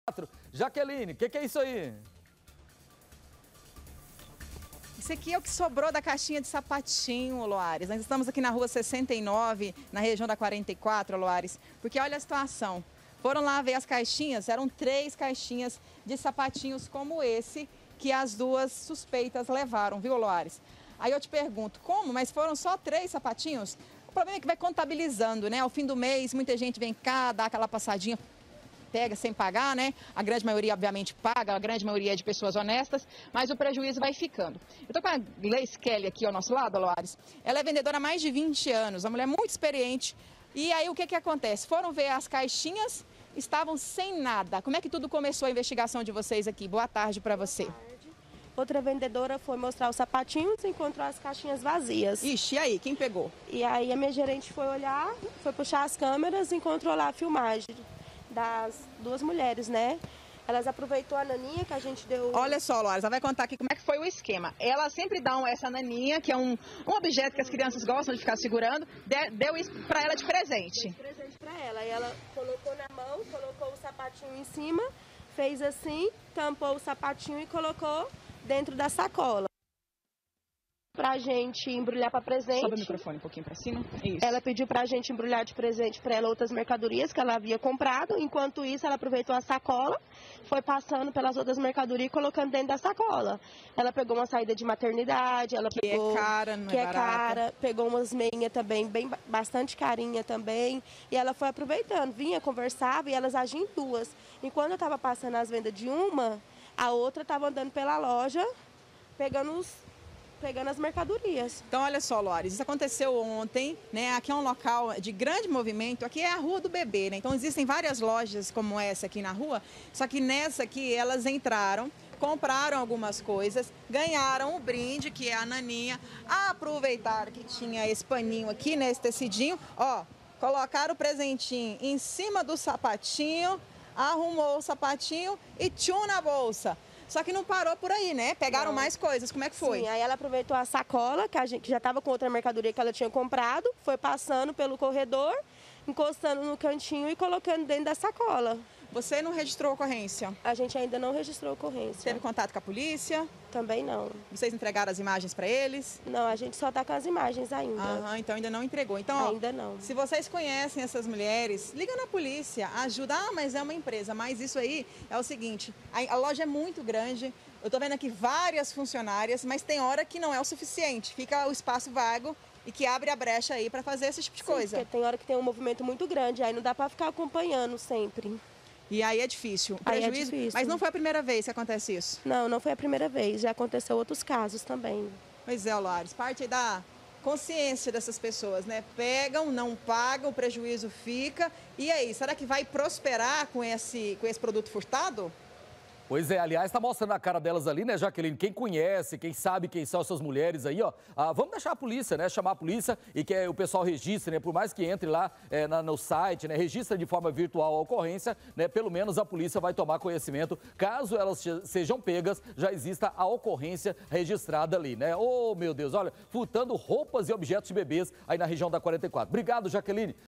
CRIME MONITORADO: CÂMERAS FLAGRAM MULHERES FURTANDO EM LOJA
CÂMERAS FLAGRAM MULHERES FURTANDO EM LOJA DA REGIÃO DA 44.